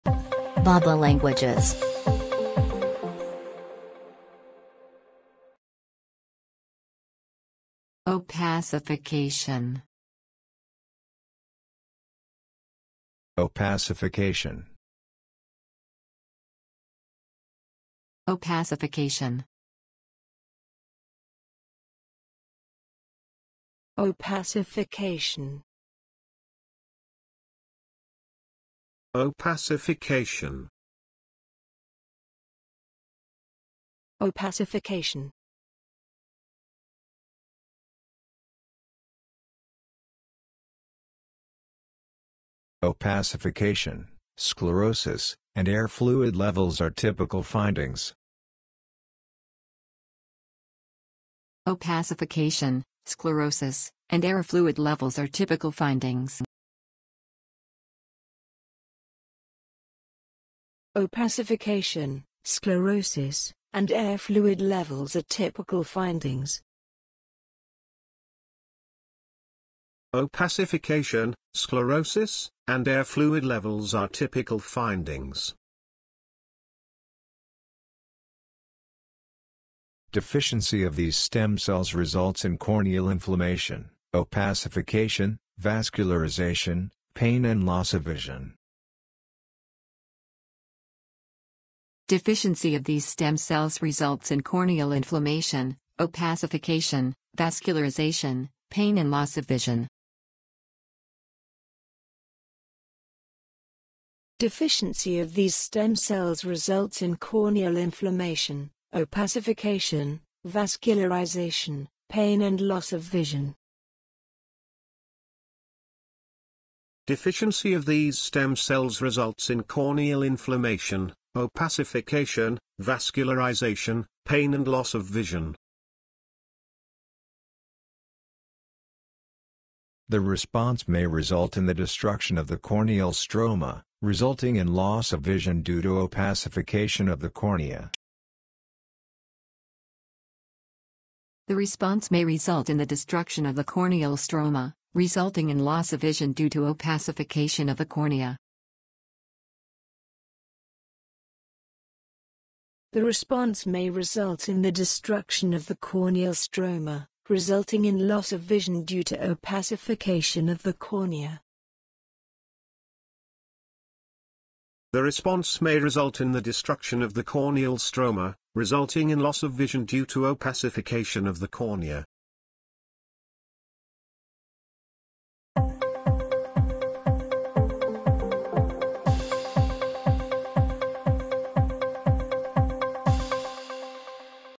OPACIFICATION pronunciation | Improve your language with bab.la
Improve your spoken English by listening to OPACIFICATION pronounced by different speakers – and in example sentences too. Learn and love languages with bab.la.